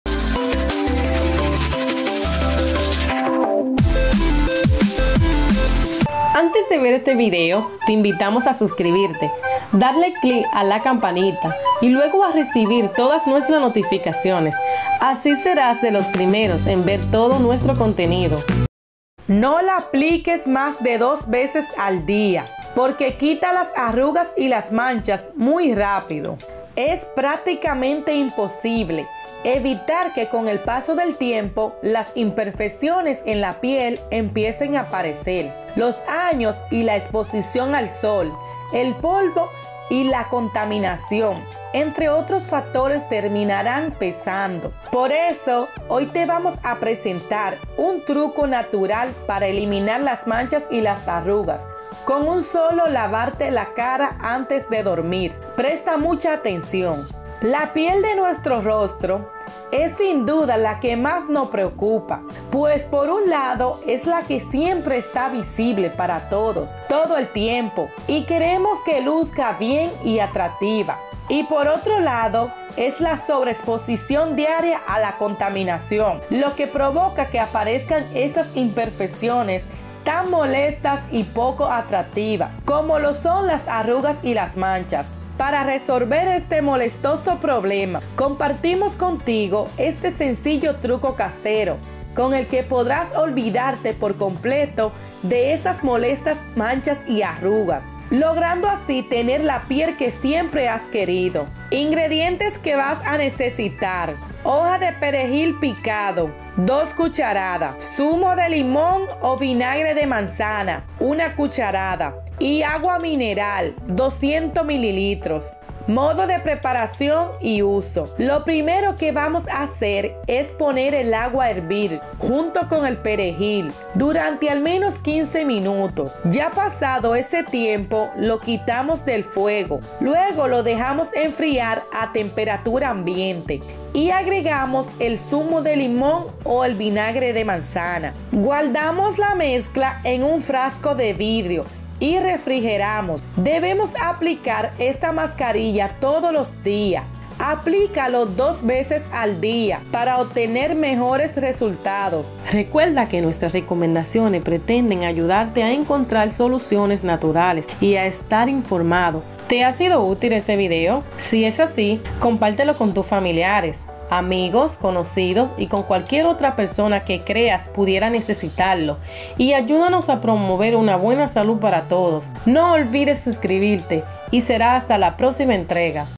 Quita las Manchas y Arrugas de Forma Rápida pero No abuses solo Úsalo 2 veces al día!
Es prácticamente imposible evitar que con el paso del tiempo las imperfecciones en la piel empiecen a aparecer. Los años y la exposición al sol, al polvo, y a la contaminación, entre otros factores terminarán pesando. Por eso, Hoy te vamos a presentar, un truco natural para eliminar las manchas y las arrugas con solo lavarte la cara antes de dormir. Presta mucha atención.

La piel de nuestro rostro, es sin duda la que más nos preocupa, pues por un lado es la que siempre está visible para todos, todo el tiempo y queremos que luzca bien, y atractiva.

Y por otro lado, es la sobre exposición diaria a la contaminación lo que provoca que aparezcan esas imperfecciones tan molestas y poco atractivas, como lo son las arrugas y las manchas.

Para resolver este molestoso problema, compartiremos contigo este sencillo truco casero con el que podrás olvidarte por completo de esas molestas manchas y arrugas, logrando así tener la piel que siempre has querido.

Compártelo así aportarás a la causa para que un amigo o una persona que necesite se beneficie de esta información importante.